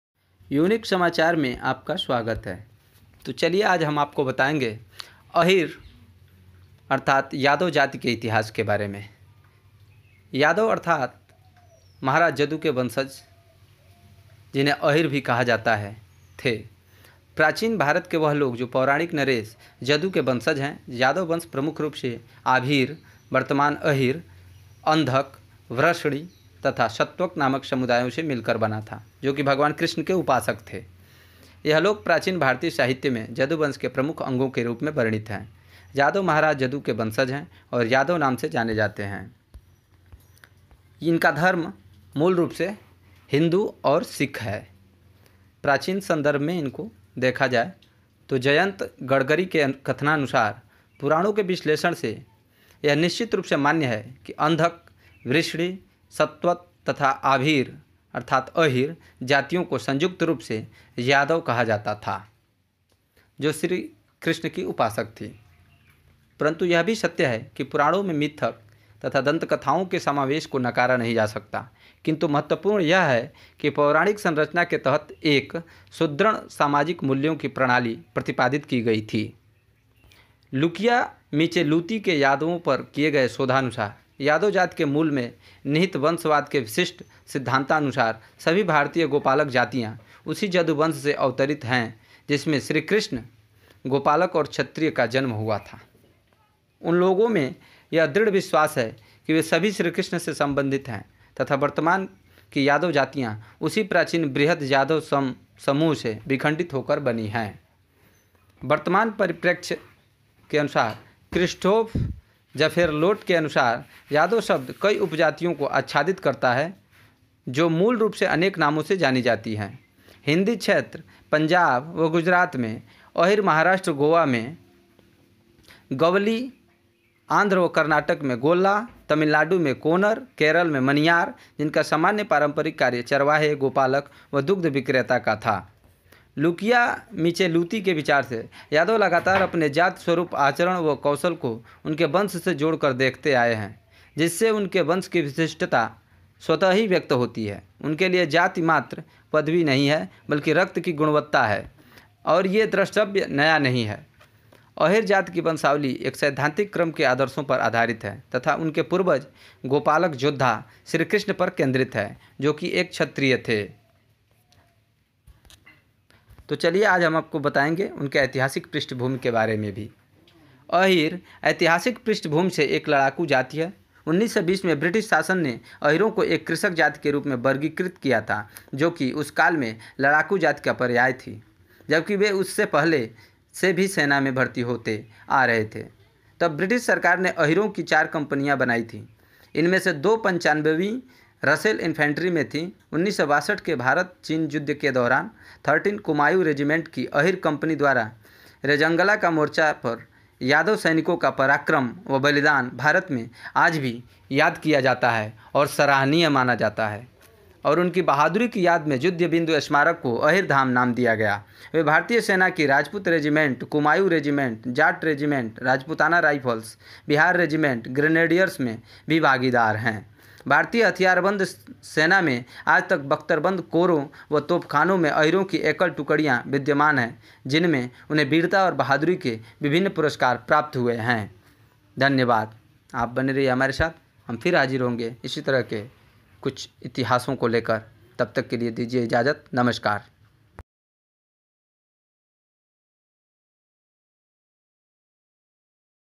यादव जाति/यदु वंश की खास बातें। वीर यादव जाति का इतिहास। history of yadava.
Unique samachar. Ahir jati ka itihas. Yadav jati ka itihas. history of yadavas. History of species/cast.